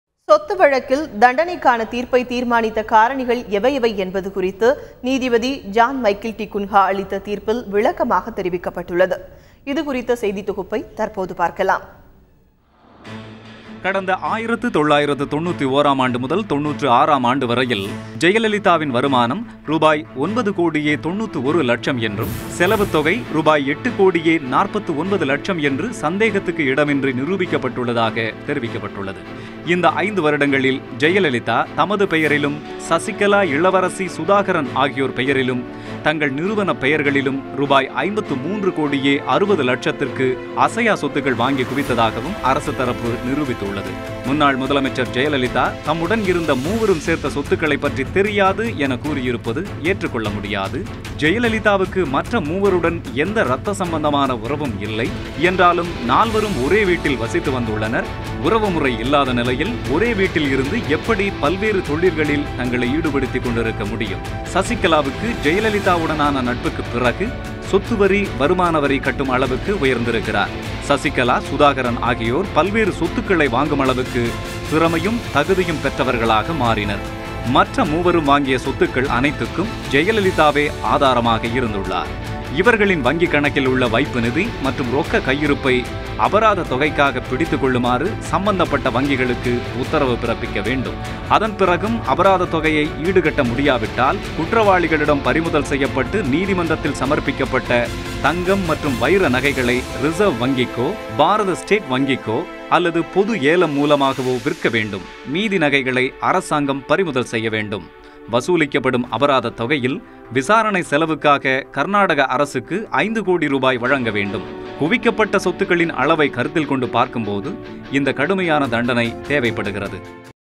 Decisive Factors in Michael D'Cunha's Final Jayalalithaa Verdict in DA Case - Thanthi TV
What were the Decisive Factors in Michael D'Cunha's Final Jayalalithaa Verdict in DA Case..? - A Special Report, Thanthi TV